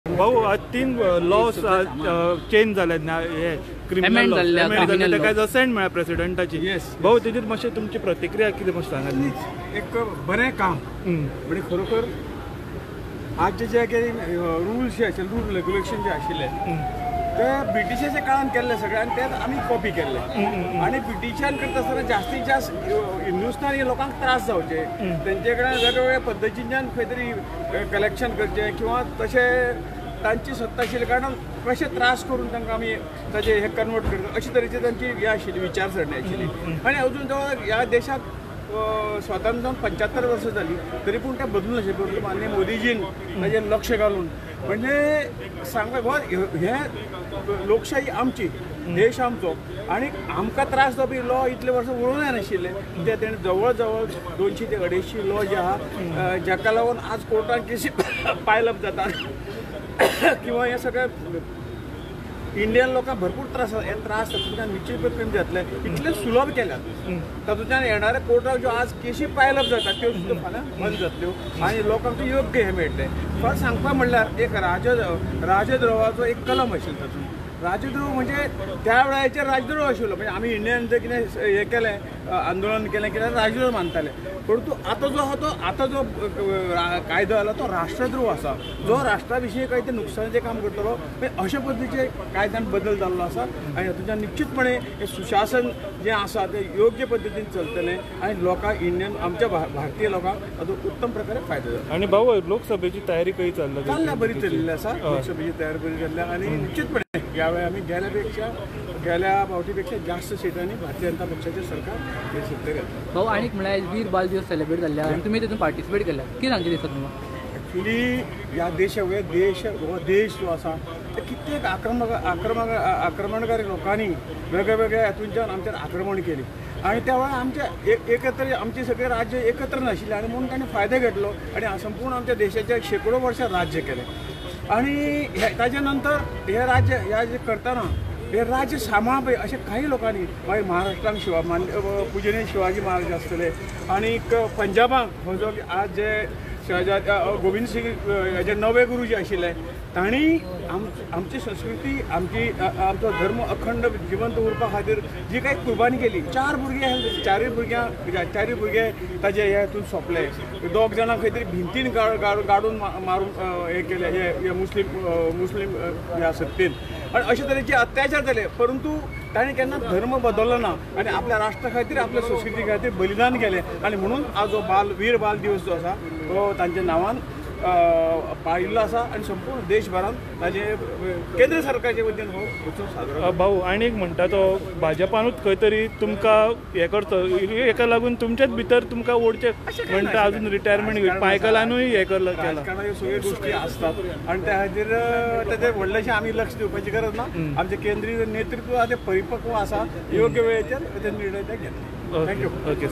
Goan Reporter: Min Shripad Naik comments on 3 Criminal Law Amendment bills & LS poll preparation
Minister of State for Tourism and Ports, Shipping and Waterways Shripad Naik comments on Government notified three Criminal Law Amendment bills and Lok Sabha poll preparation 2024.